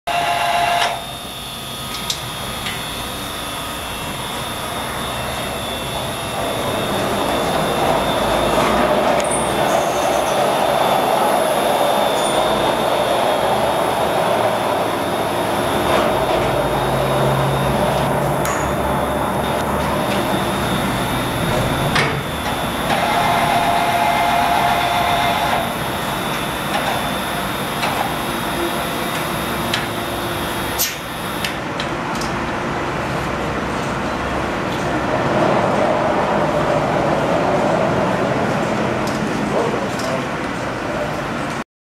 Lahko mostno dvigalo z vakuumskim prijemalom za steklo by Indenna
V videu si lahko ogledate lahko mostno dvigalo 500 kg na ročni pomik z električnim verižnim vitlom ter vakuumskim prijemalom za transport stekla. Proga dvigala je obešena na stropne betonske preklade, s tem smo se izognili stebrom v prostoru. Dvižna rešitev by INDENNA.